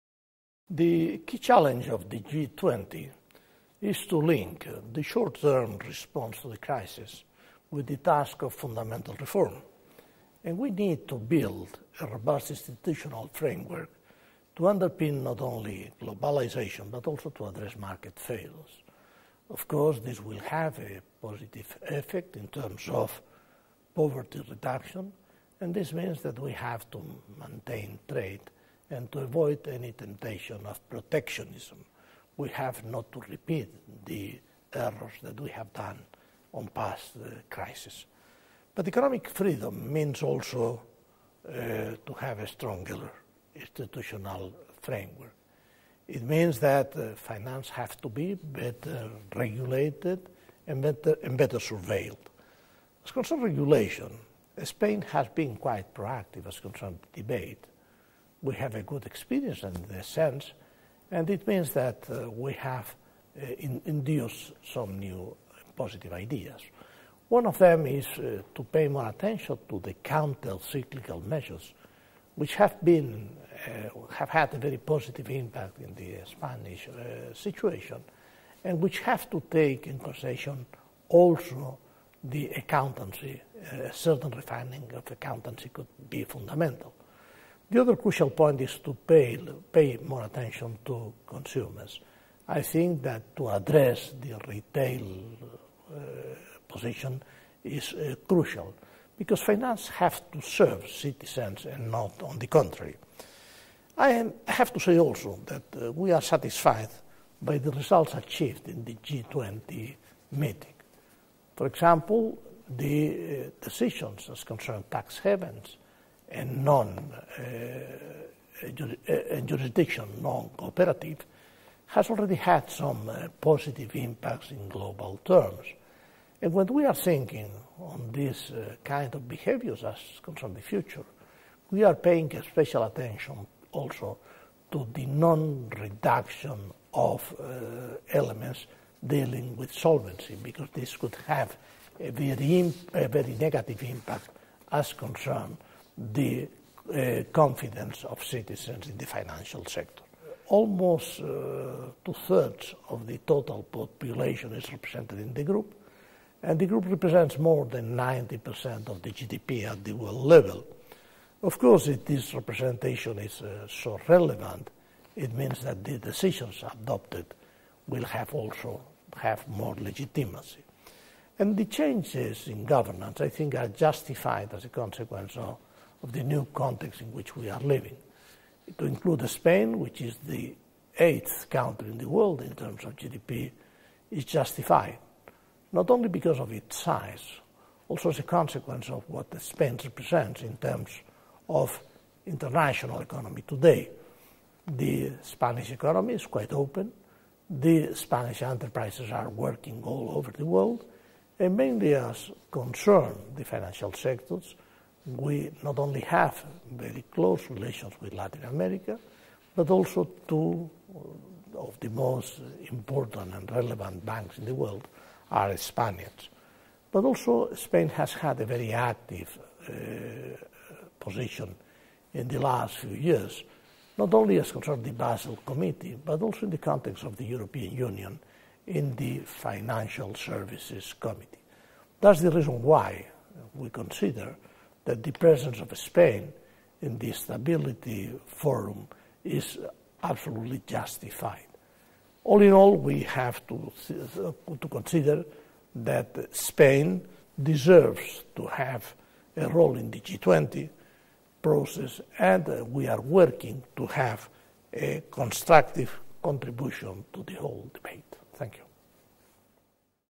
Spanish Vice-President and Minister for Finance and the Economy, Pedro Solbes
'The key challenge of the G20 is to link the short-term response to the crisis with the task of fundamental reform. We need to build a robust institutional framework to underpin, not only globalisation, but also to address market failures.'